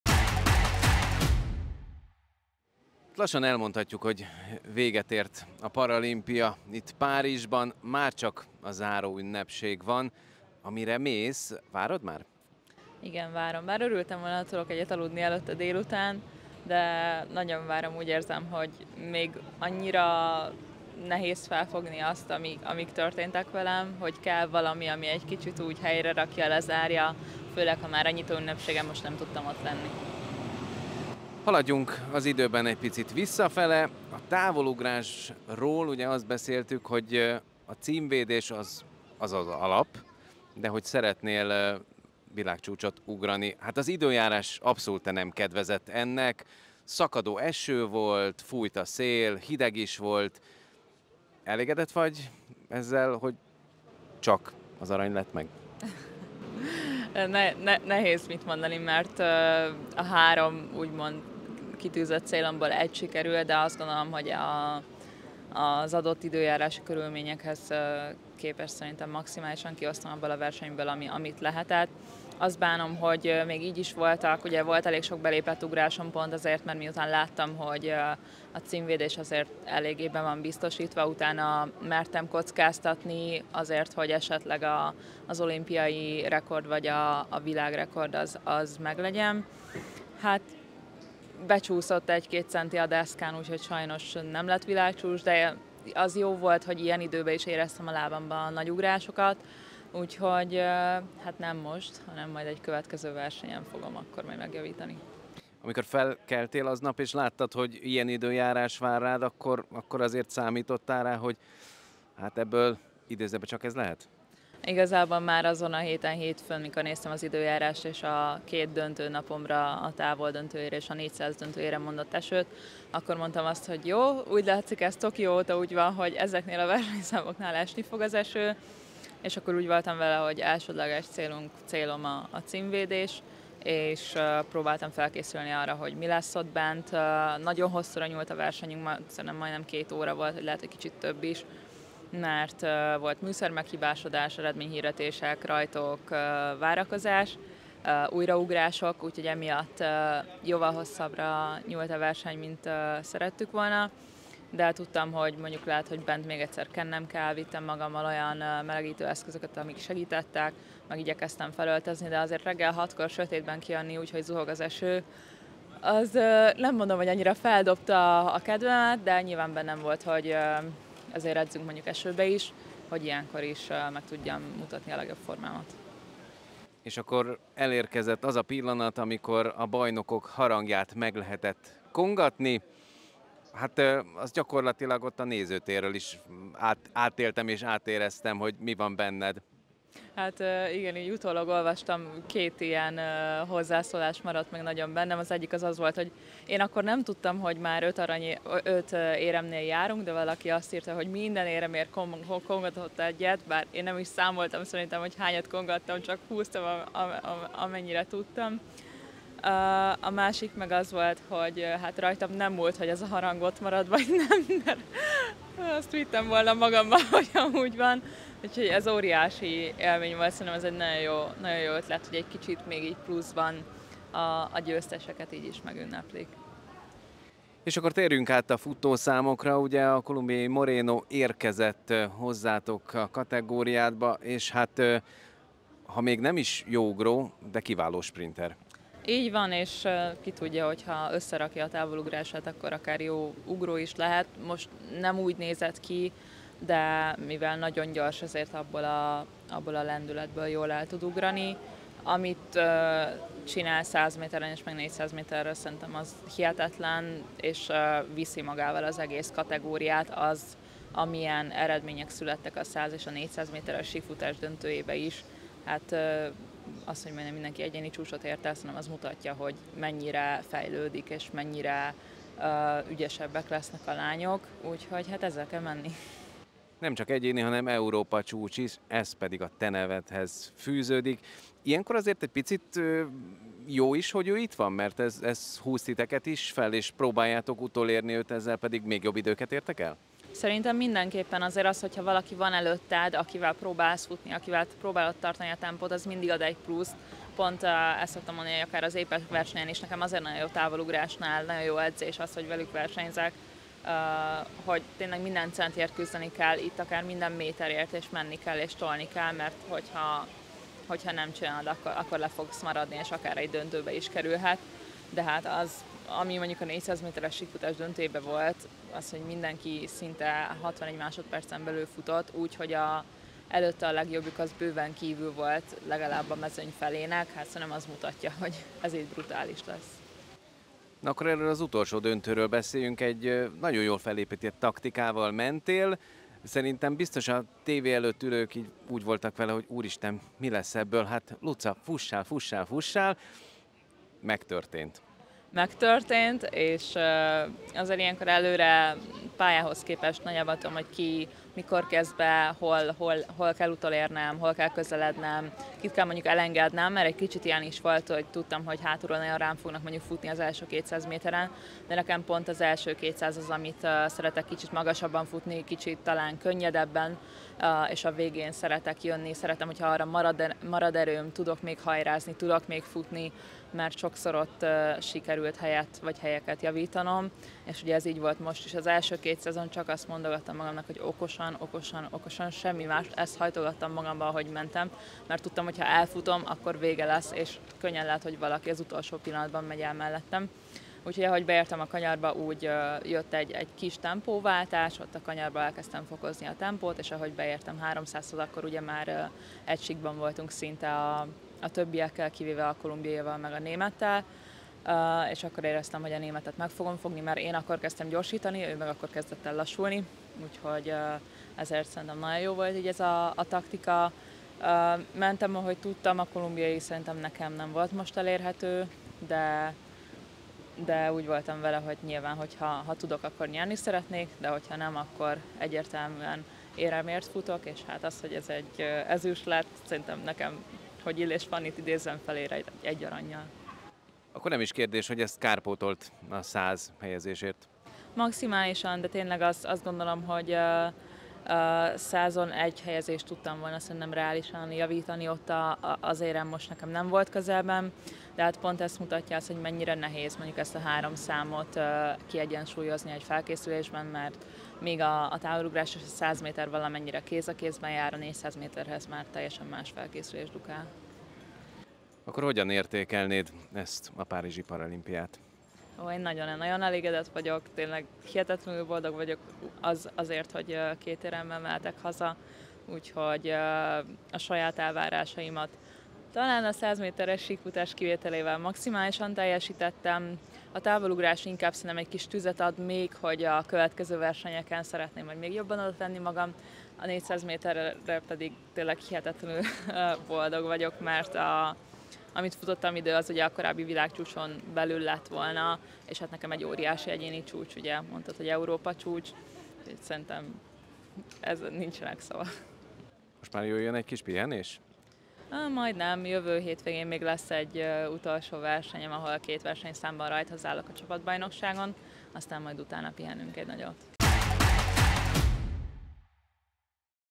Ekler Luca: A saját elvárásaimat maximálisan teljesítettem
A teltházas Stade de France-ban három számban versenyzett Ekler Luca, aki a T38-as kategóriában a távolugrás döntőjét magabiztosan nyerte meg, ezzel megvédte Tokióban szerzett címét, majd a 400 méteres síkfutás döntőjében egy taktikus versennyel felállhatott a dobogó második fokára. A TFSE 25 éves atlétája Párizsban értékelte második paralimpiáját a Sportal Tv stábjának.